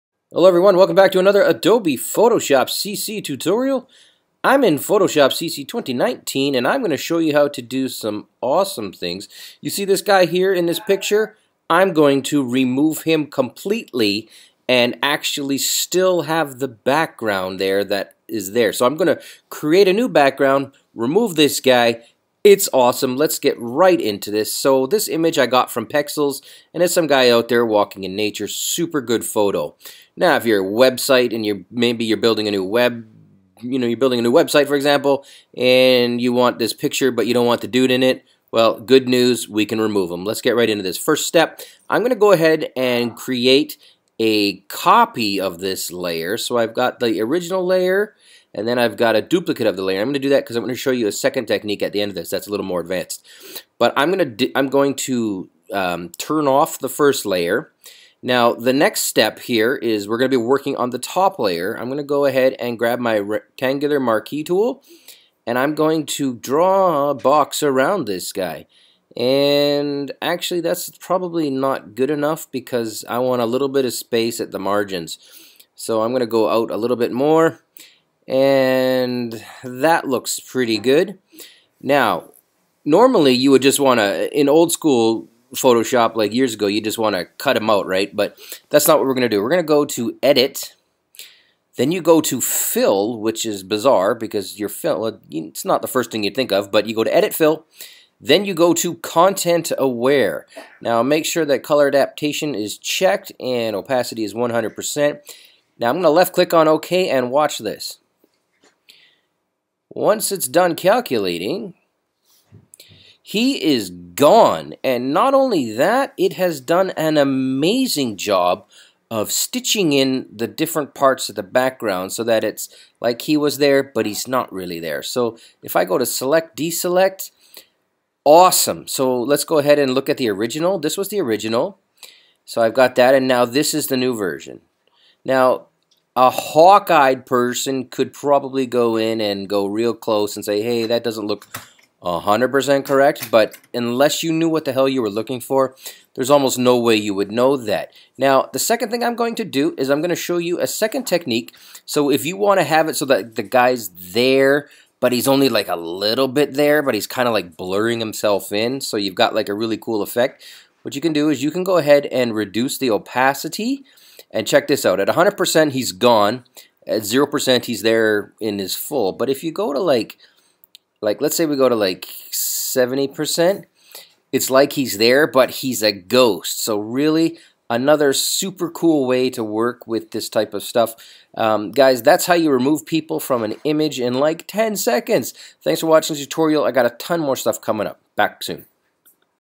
How to remove objects in photoshop CC
In this tutorial, I show you how to remove objects and people in photoshop cc 2019.  We use the content aware fill functionality primarily with a ghost effect shown at the end for good measure.